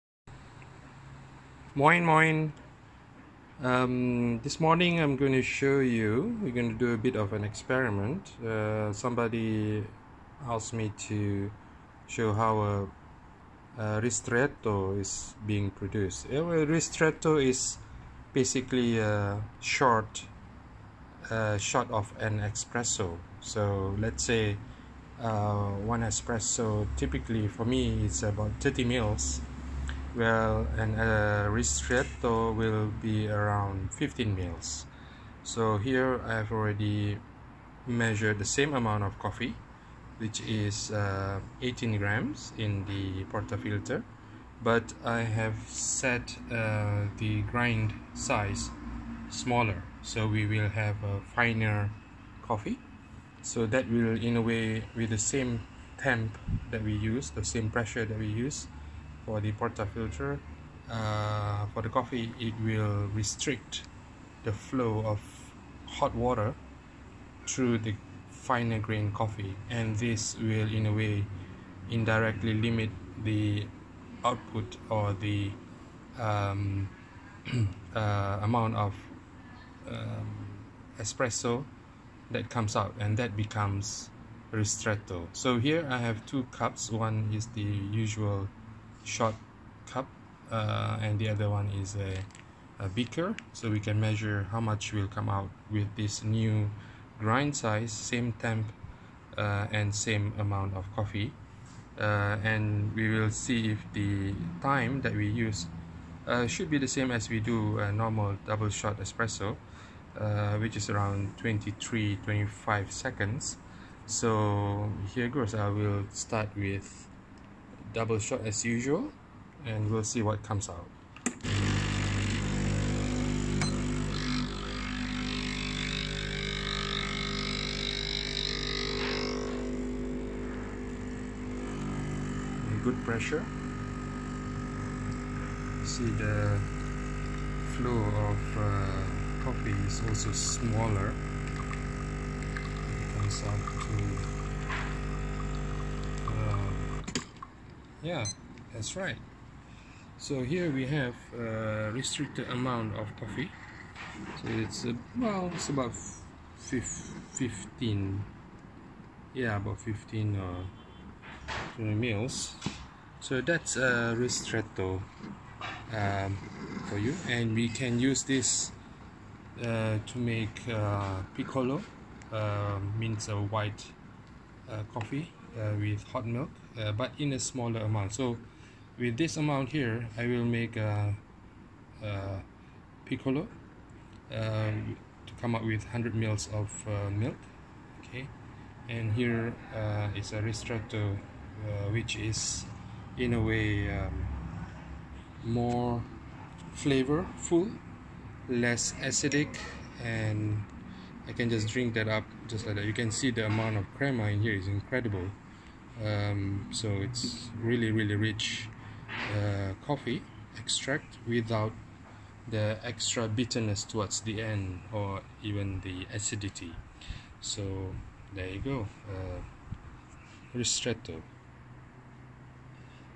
Ristretto @ Café de Balcony KL
The difference between a Ristretto and an Espresso